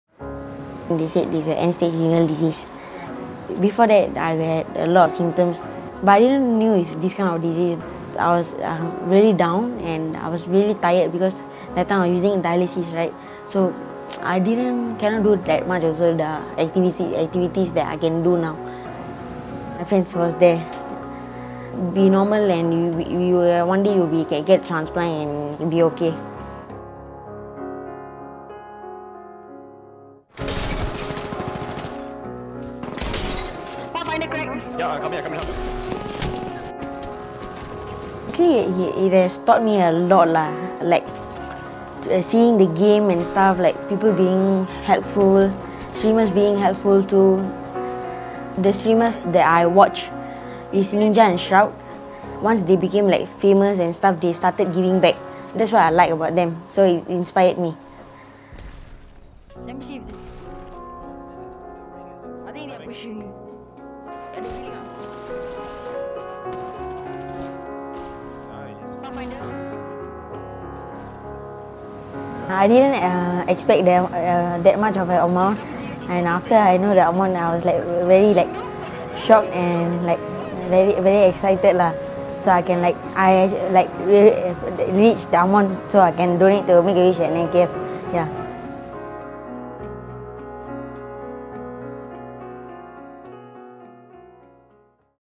Wishing to Give Back
15-year-old Shakeel was diagnosed with end stage renal disease but recently received a kidney transplant.
Watch the video to find out how he turned a wish from the Make-A-Wish Foundation into one that gave back. Video: Najeer Yusof/TODAY